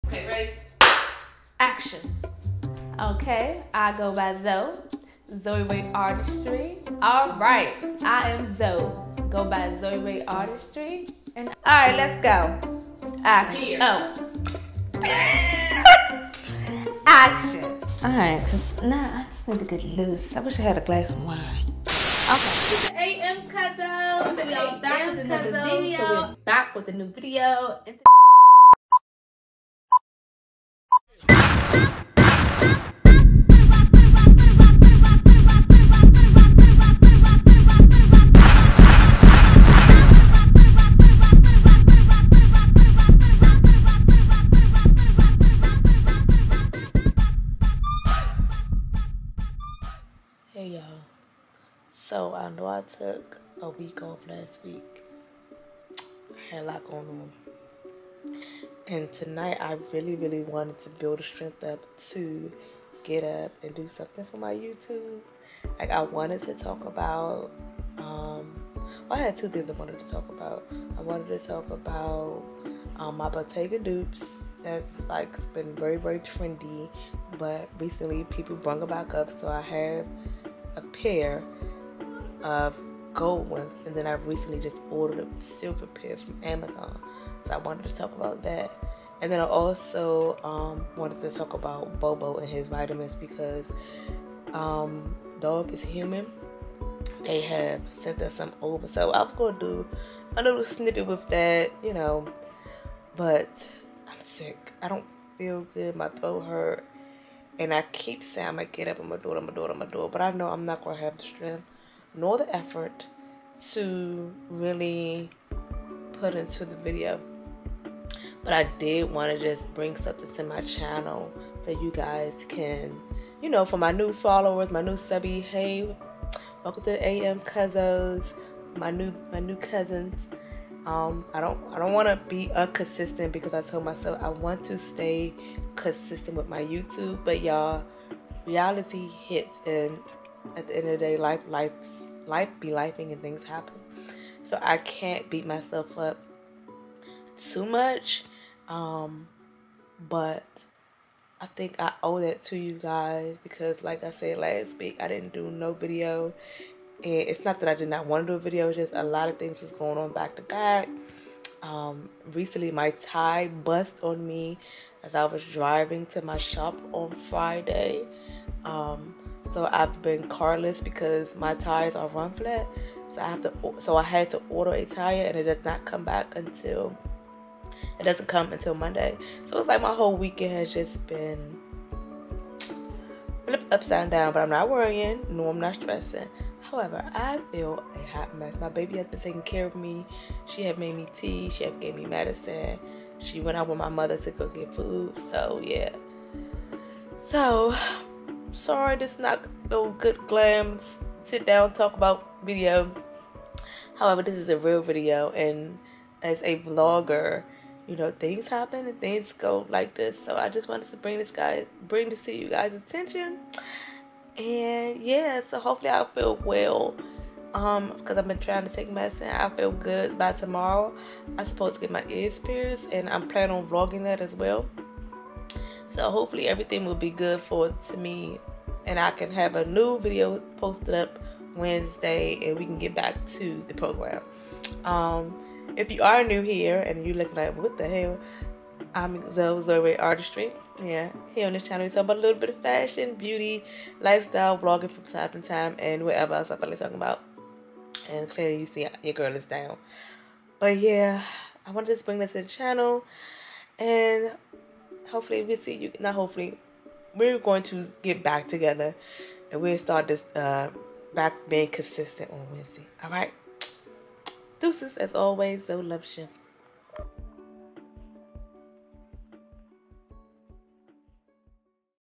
I OWE YOU ALL AN APOLOGY!
WELCOME TO THE AM CUZO!

You girl has been down. Last week so much was going on I didn’t put my best feet forward with my YouTube and now I’m sick. I told myself I want to stay consistent ￼and I feel like I failed you guys. So here my apology and baby I’m back and ready to bless my Cuzo with some fun, fashionable and REAL, RAW, UNCUT!

As always Zoe Luv Ya!

Equipment
 iPhone 13 Pro
Sony ZV1

PR package: 3100 Ritchie Rd, District Height MD 20747